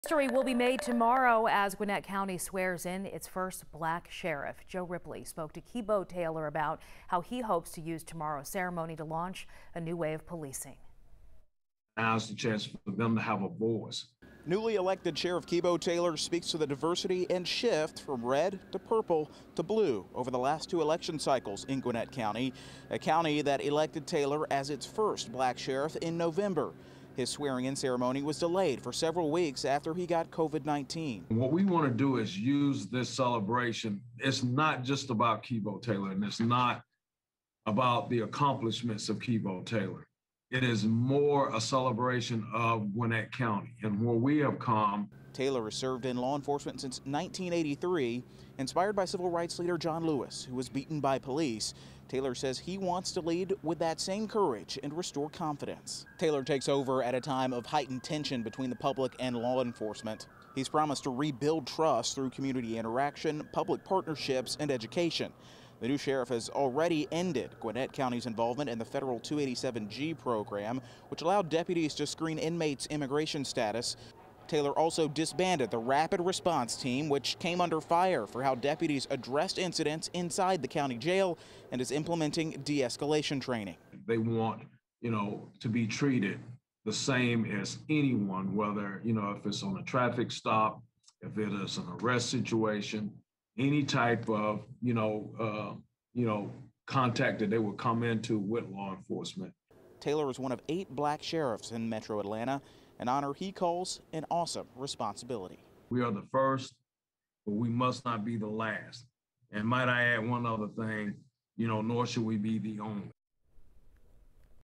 Gwinnett County's first black sheriff reflects on inspiration during Black History Month
Keybo Taylor plans to use the swearing-in ceremony to launch a new way of policing.